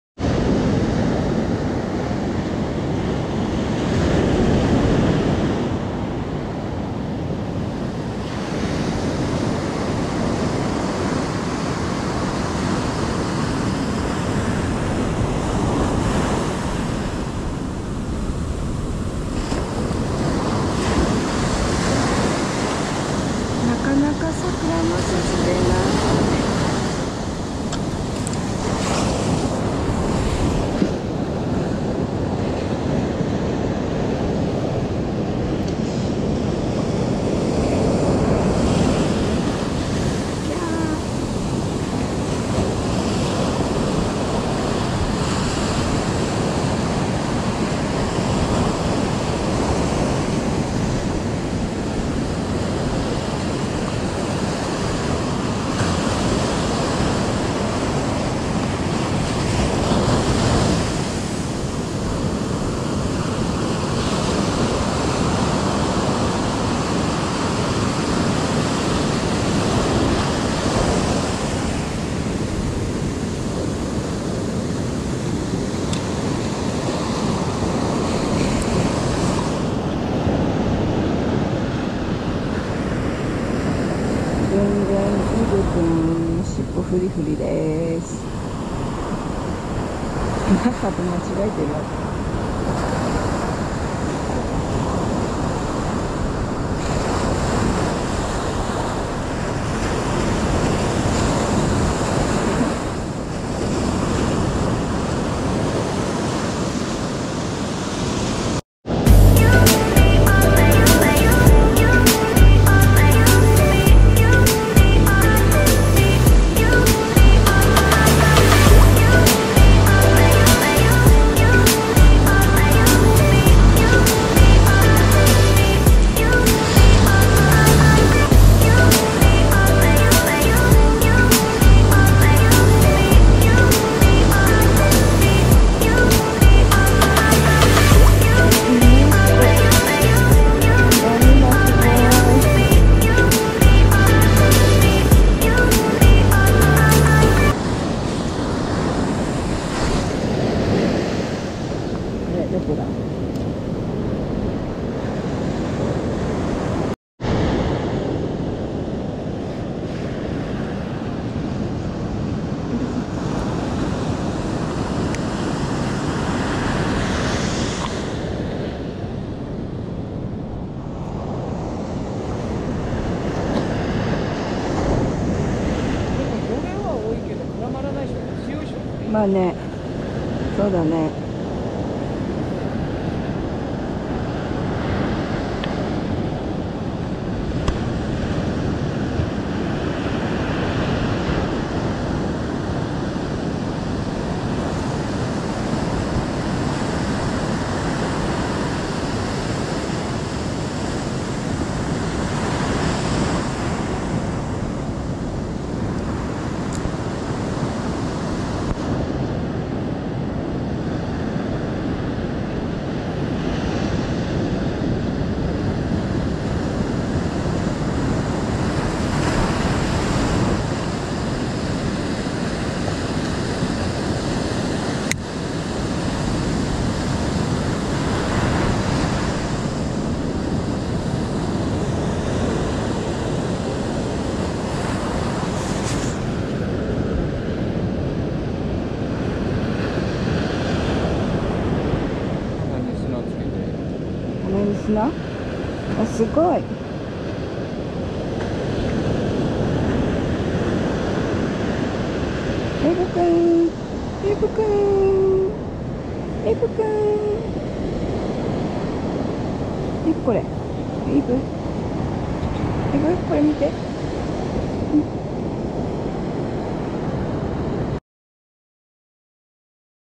5/4🌸サクラマス求めて
🤣今日も🌸サクラマス 釣れませんでした
ボウズで終わりました